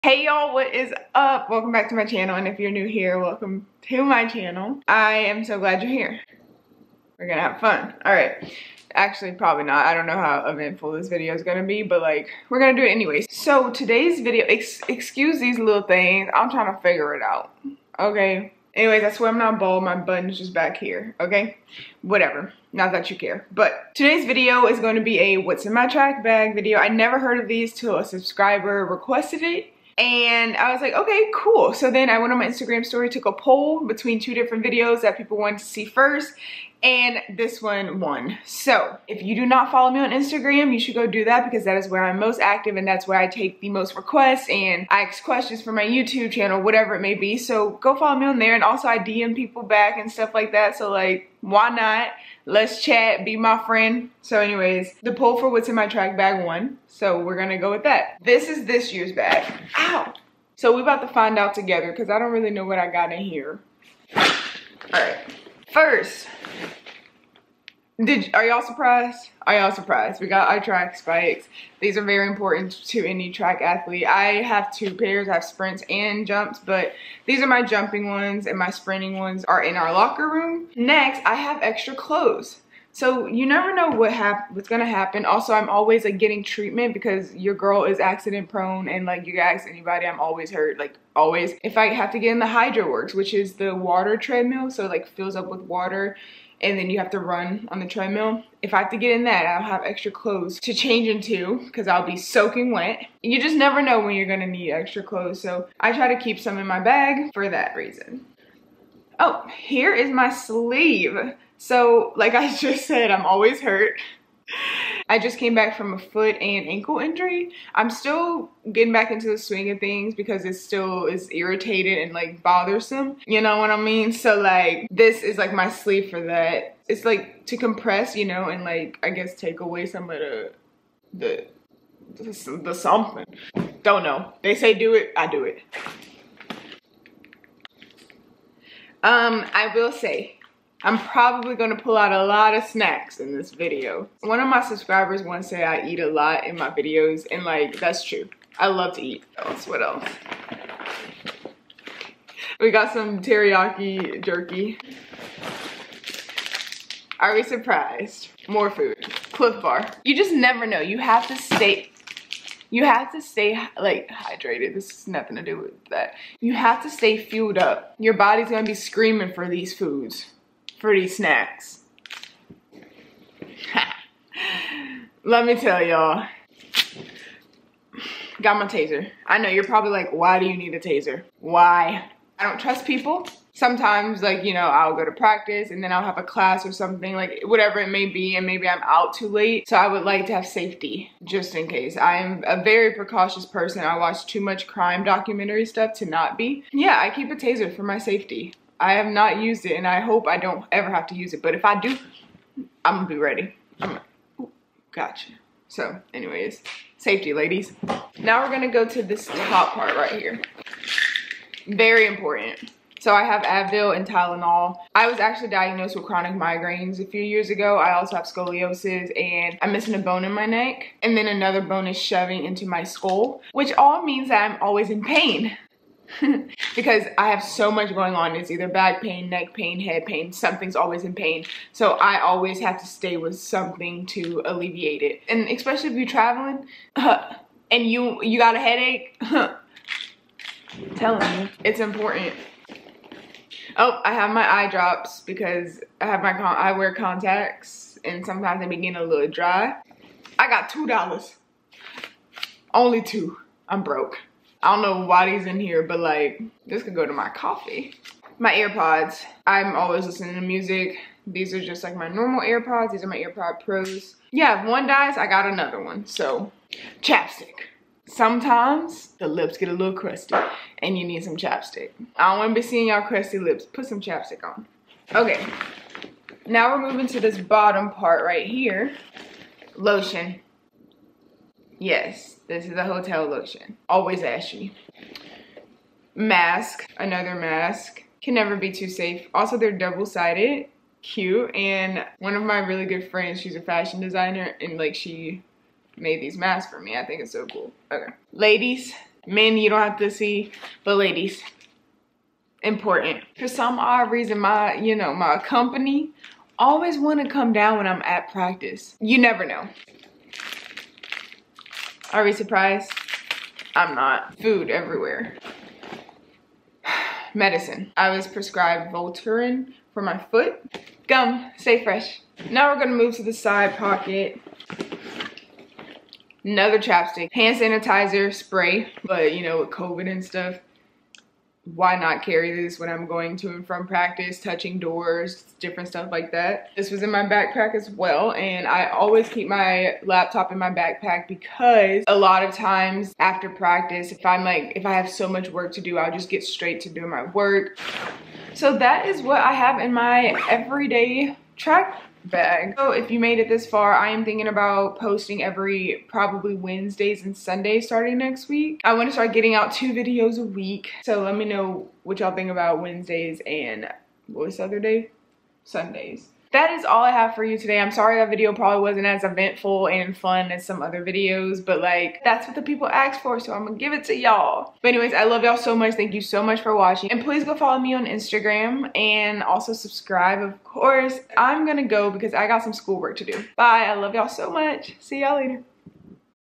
WHATS IN MY TRACK BAG?????
A subscriber recently requested a "what's in my track bag" video, so here it is!!! Here are all of the things I typically have in my track bag that just makes sense ;) What do you carry in your track bag?
LETS BE FRIENDS :)
SNAP: @jalaiyaaaa
Thank you for taking time to watch this video, you are the best!!